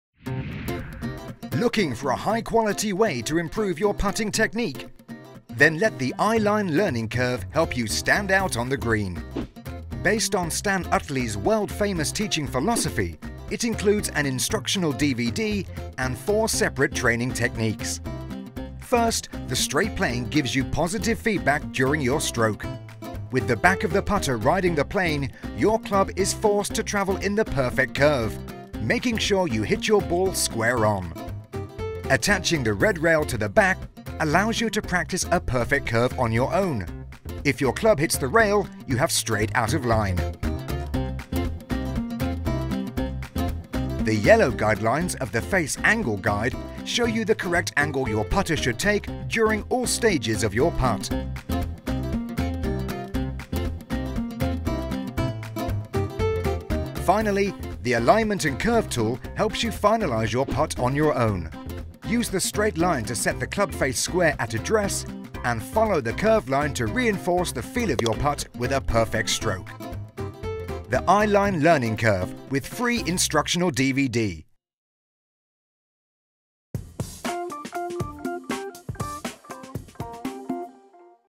Masters Golf - Eyeline Learning Curve (PE142)
Designed to support Stan Utley's teaching philosophy, the Learning Curve System will condition you to be more comfortable, relaxed and athletic on the green. You'll develop the fundamentals of a free-flowing putting stroke as Utley himself teaches you to use the Learning Curve in any of several ways on the accompanying DVD.